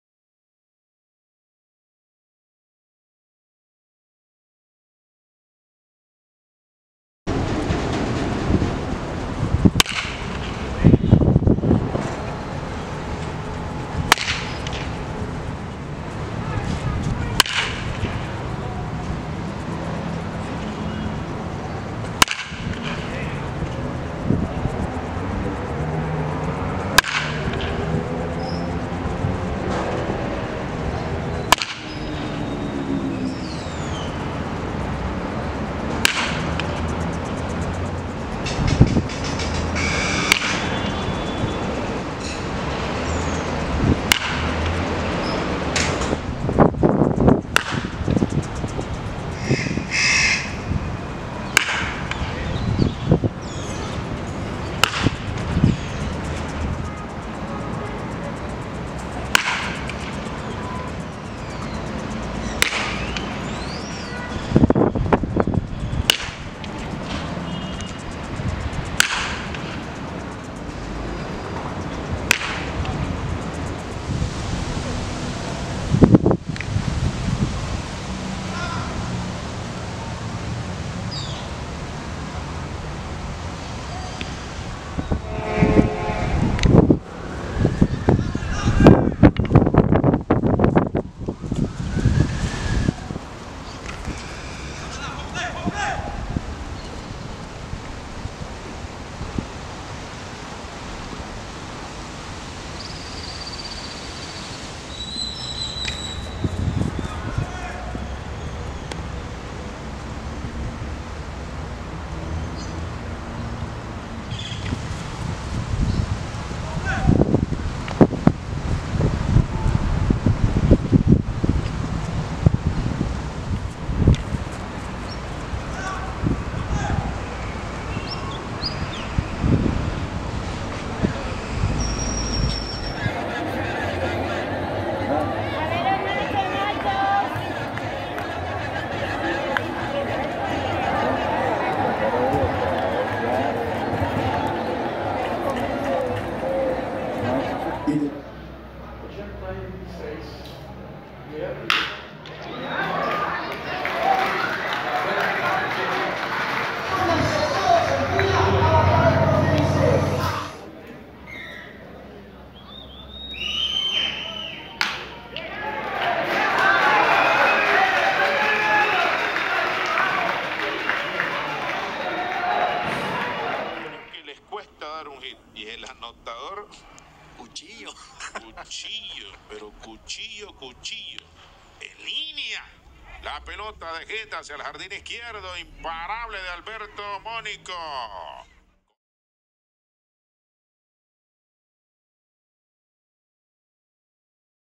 TPF - Hector Rodriguez - Outfielder - Class of 2021Recruiting Video
Hector is the an OF from the El Salvador National Team